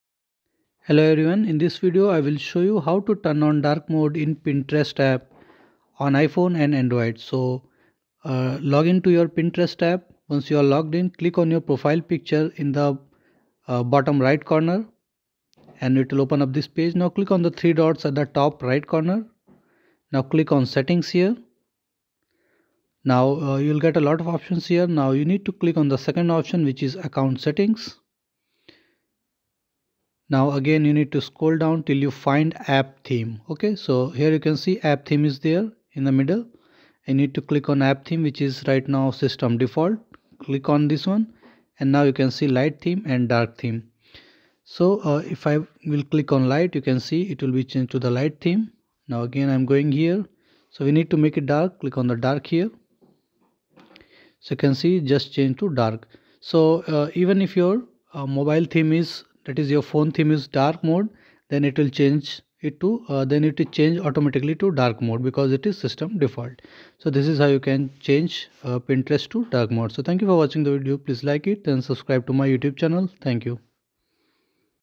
How To Turn On Dark Mode in Pinterest App (2022)
Pinterest DarkMode Learn how to turn on dark mode theme in Pinterest app on Android and iPhone.